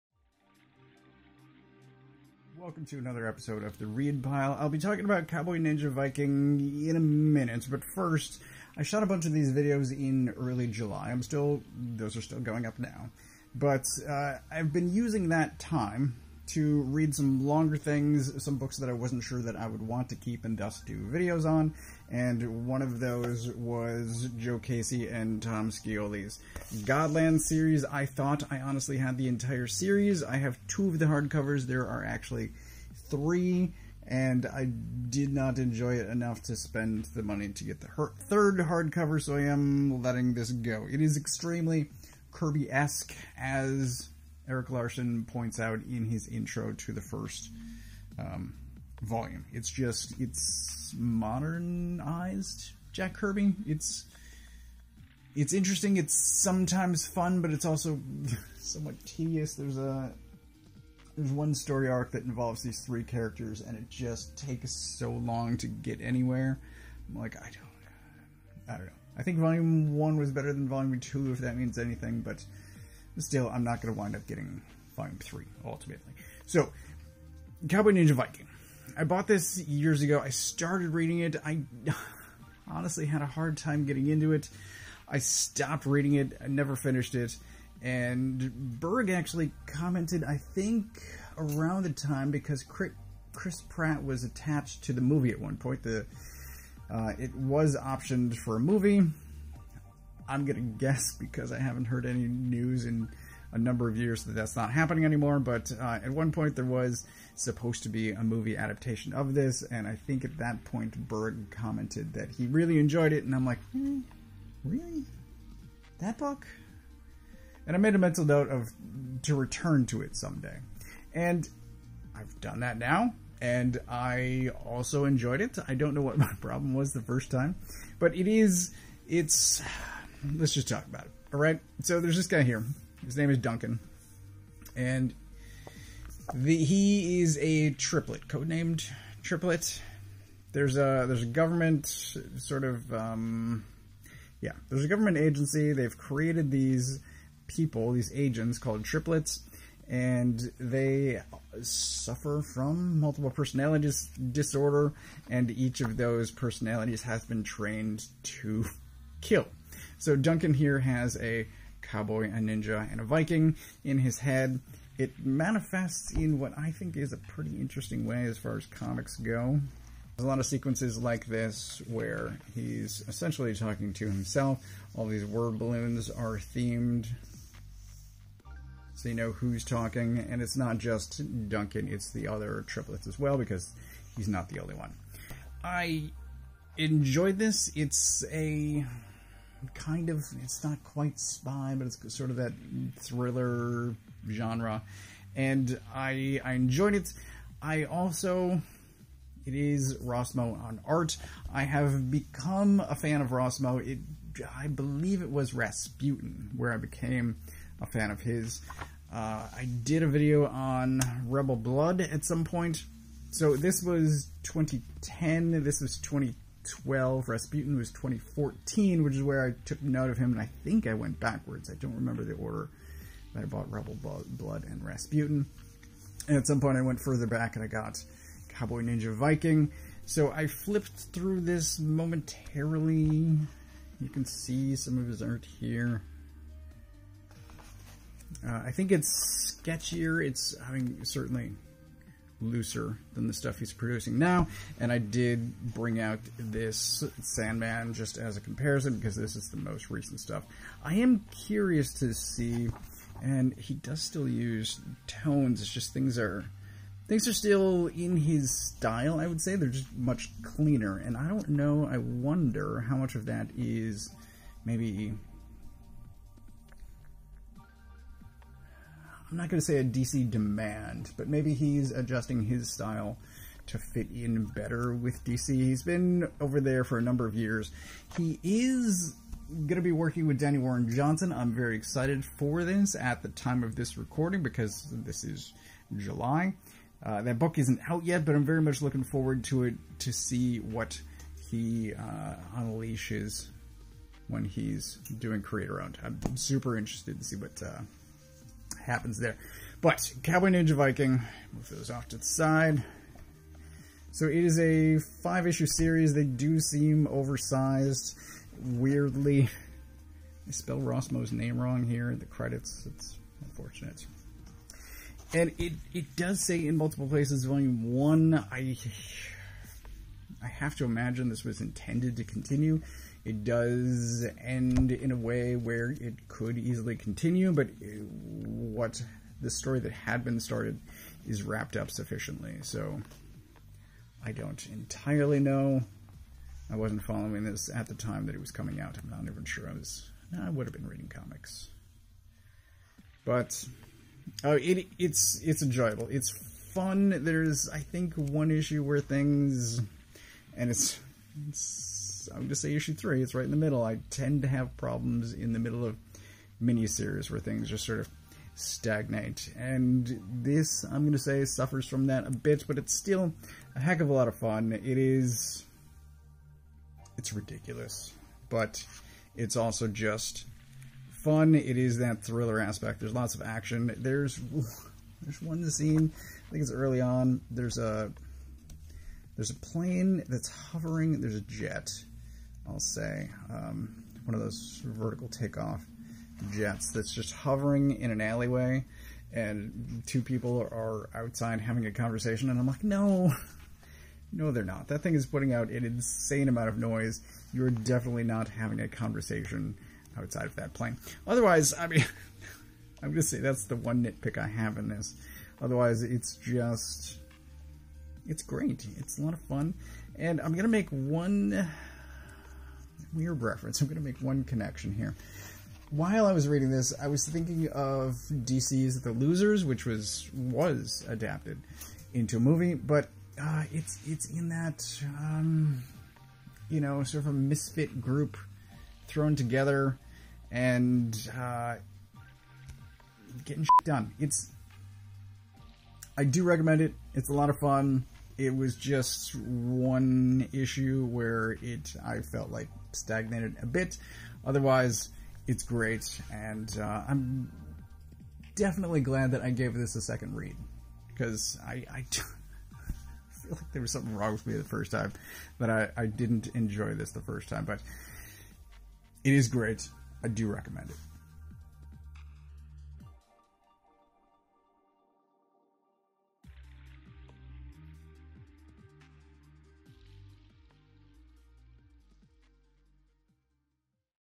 The READ PILE: "COWBOY NINJA VIKING Vol. 1" - Comic Review
Toast takes a look at "Cowboy Ninja Viking, Vol. 1" written by  A. J. Lieberman, with art by Riley Rossmo. It was published by Image Comics.

"International intrigue! Black-market nukes! And Cowboy Ninja Viking! Sent on a covert mission that will take the triplets from Bangkok to the Middle East, Duncan, Ghislain, Nix, Grear, Steed Malbranque, and everyone's favorite gladiator/pirate/oceanographer Yashitiko Ammo are gonna save the world... or totally screw it up!"

If you would like to find a comic shop near you, please use the following URL: